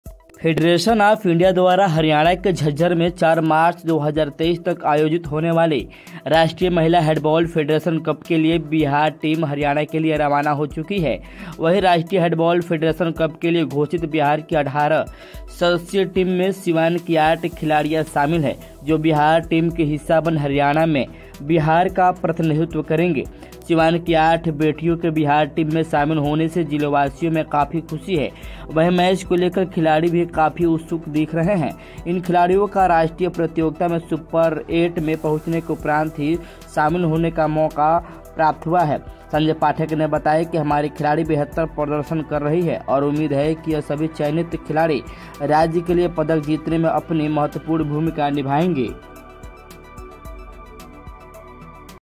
राष्ट्रीय महिला हैंडबॉल फेडरेशन कप में खेलने के लिए बिहार की टीम झज्जर रवाना, टीम में सीवान की 8 महि
जिला हैंडबॉल संघ के सचिव एवं रानी लक्ष्मीबाई स्पोर्ट्स एकेडमी के संस्थापक संजय पाठक ने बताया कि बिहार राज्य महिला हैंडबॉल टीम की घोषित 18 सदस्यीय टीम में रानी लक्ष्मीबाई स्पोर्ट्स एकेडमी मैरवा की 8 बेटियां शामिल है और अपनी टीम के साथ झज्जर के लिए रवाना हो चुकी है. इन टीमों का चयन राष्ट्रीय प्रतियोगिता में सुपर 8 में पहुंचने के उपरांत किया जाता है.